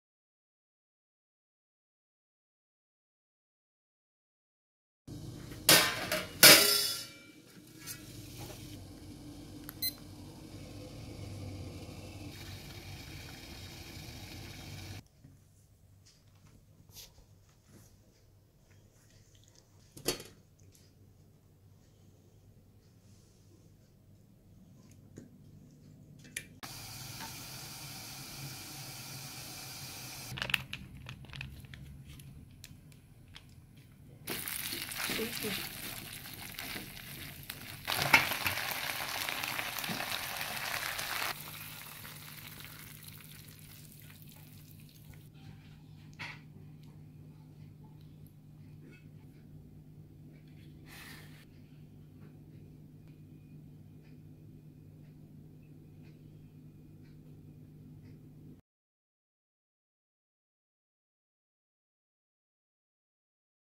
Sambal Chili Steam Cod with MasFood
Make your own sambal chili shrimp steamed cod with MasFood's Sambal Chili Shrimp All Purpose Paste, 180g per packet, halal, £3 each

We're based in the UK... and deliver all around the UK, Europe, Ireland and beyond. Thank you for choosing Niclaw Gourmet~!